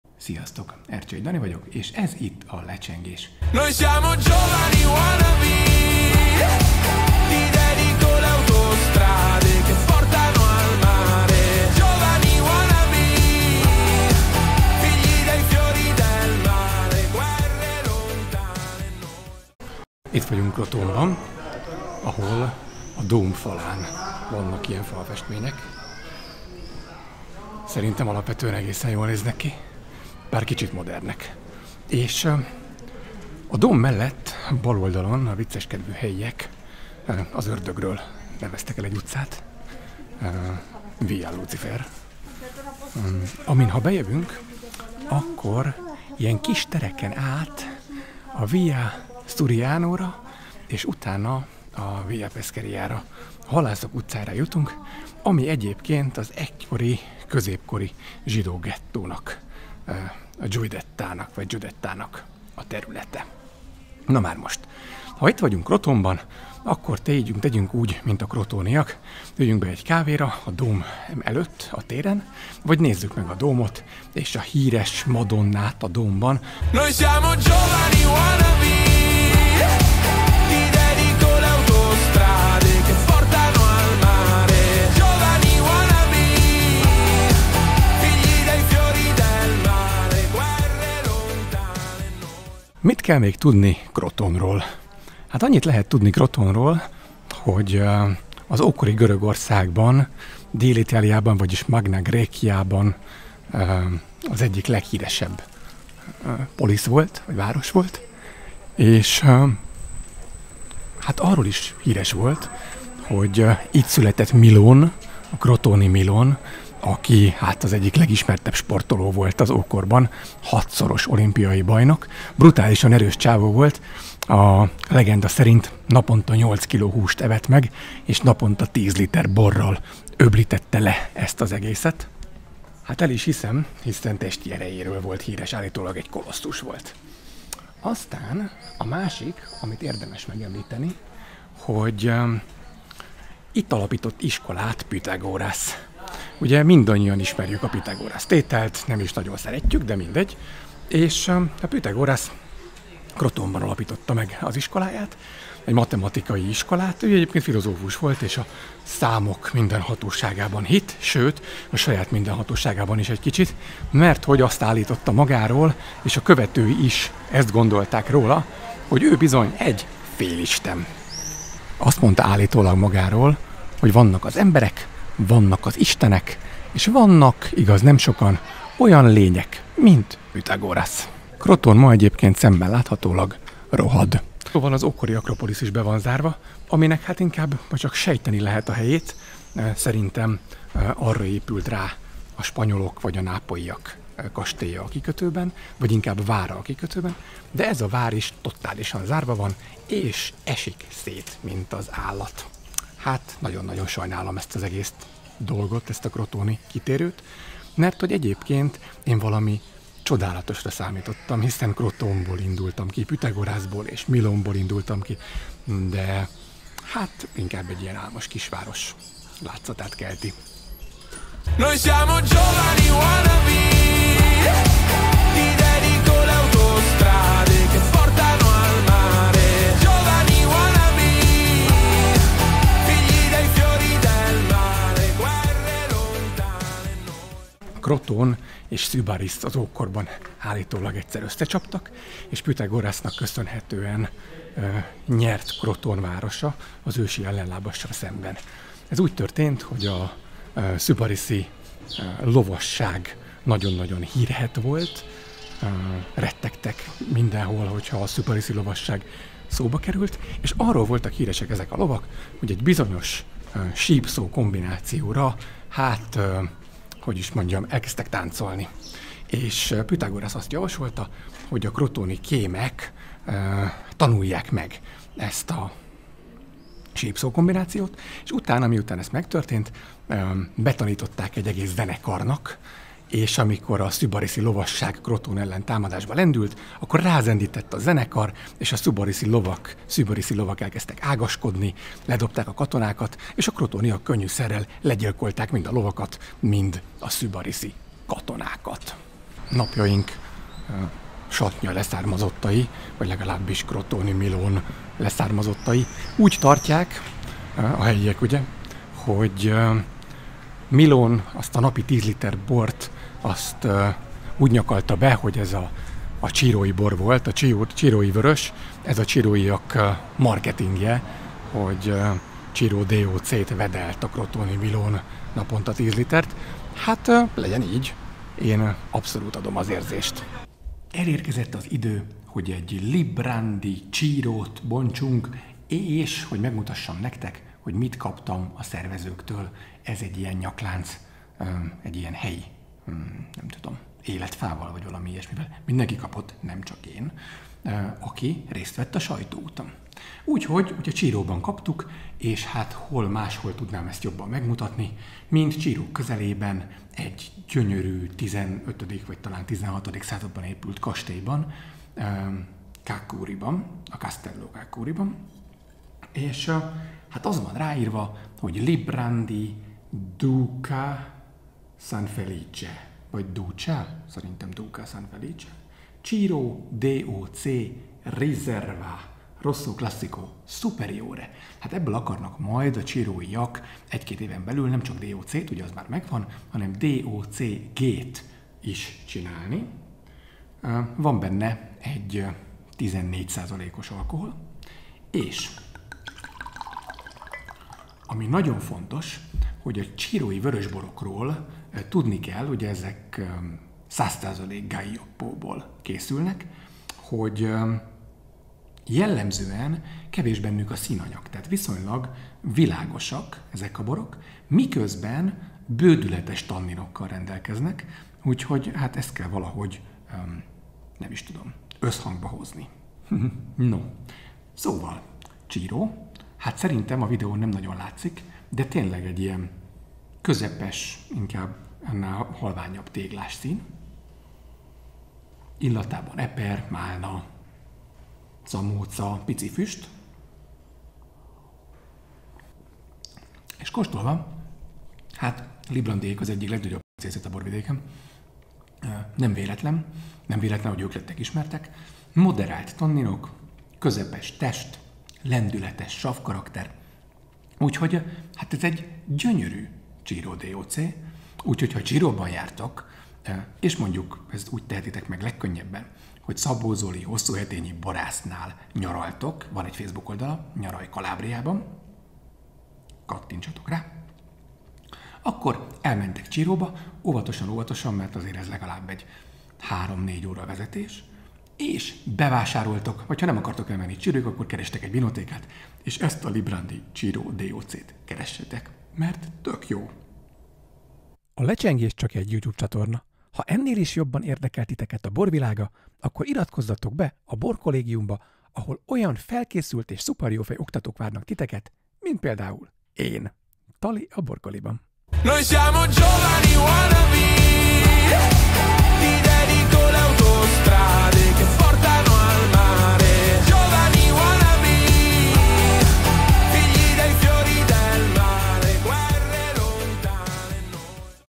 A filozófus félisten és az olimpiai bajnok Krotónban
Crotone Olaszország déli részén, a csizma csücskén, Calabriában található, nem messze a Ciró DOC-től. Ahogy a videóból is kiderül, itt alkotott Püthagorasz és itt élt az izomkolosszus és nagyivó Milón, de ettől még napjaink Crotone-ja visszafordíthatatlanul elrohad. Szóval nézzétek meg amíg még nem késő és igyatok a nagy ijedségre egy pohár cirói vöröset, mint én is.

Ercsey Dani vagyok, a Pécsi Borozó szerkesztője, a WineSofa főszerkesztője, nemzetközileg is ismert borszakíró. Köszi!